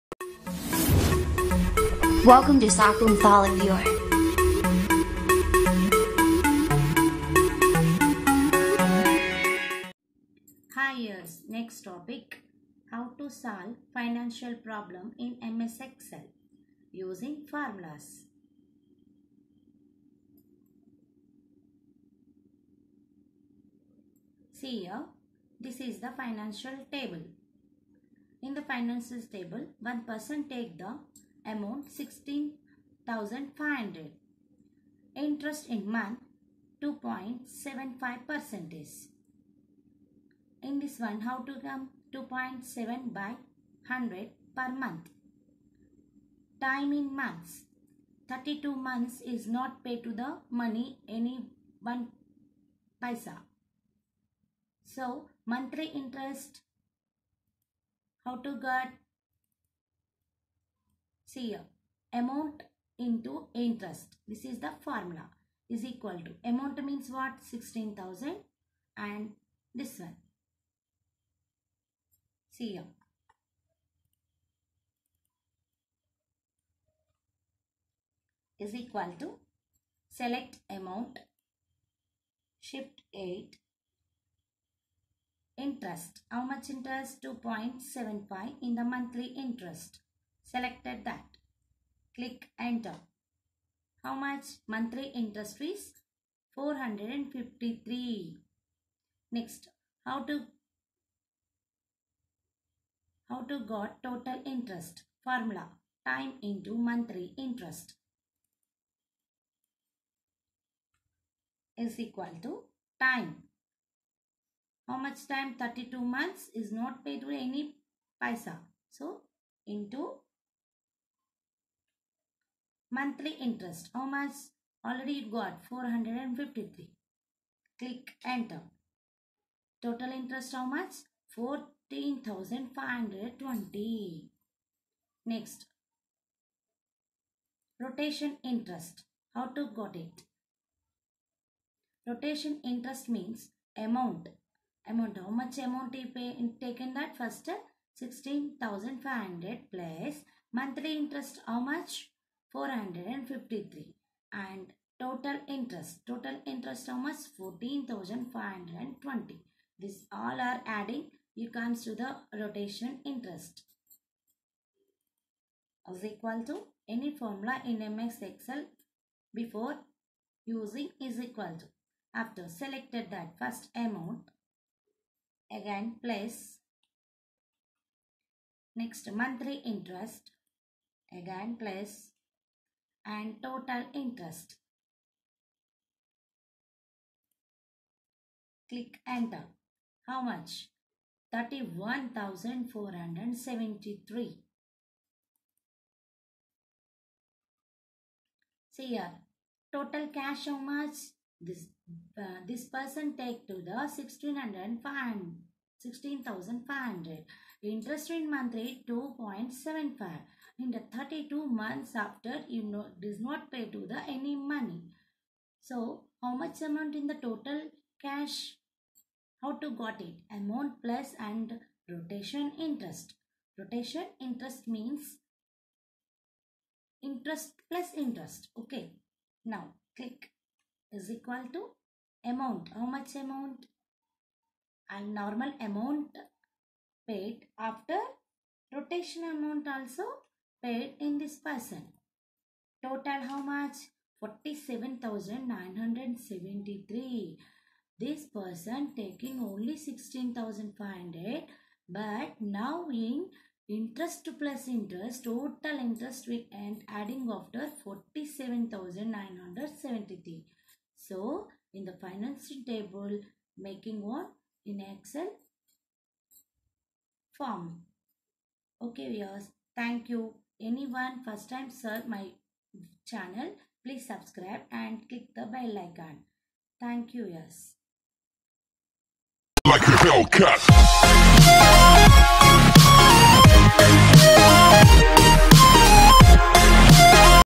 HOW TO SOLVE FINANCIAL PROBLEM IN MS EXCEL USING FORMULAS
IN MY CHANNEL MS- OFFICE AND CTET  AND MATHS 1ST TO 10 TH CLASSES